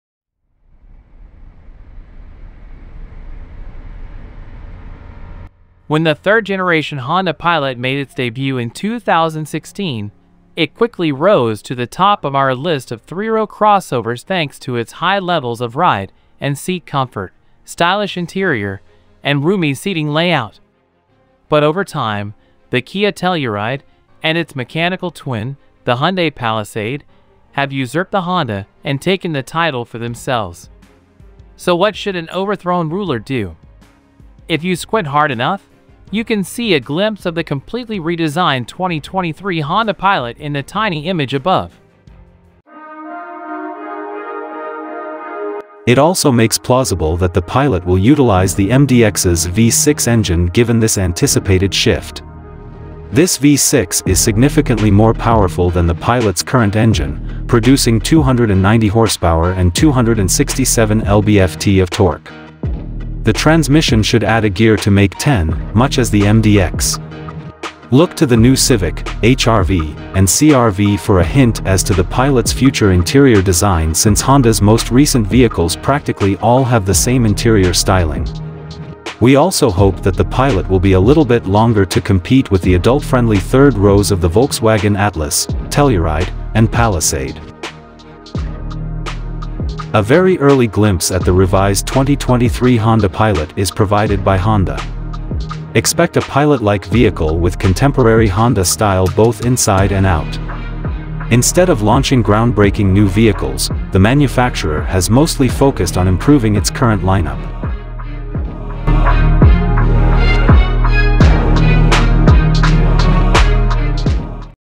2023- 2024 Honda pilot Redesign - New Information!
When the third-generation Honda Pilot made its debut in 2016, it quickly rose to the top of our list of three-row crossovers thanks to its high levels of ride and seat comfort, stylish interior, and roomy seating layout. But over time, the Kia Telluride (and its mechanical twin, the Hyundai Palisade) have usurped the Honda and taken the title for themselves. So what should an overthrown ruler do? If you squint hard enough, you can see a glimpse of the completely redesigned 2023 Honda Pilot in the tiny image above.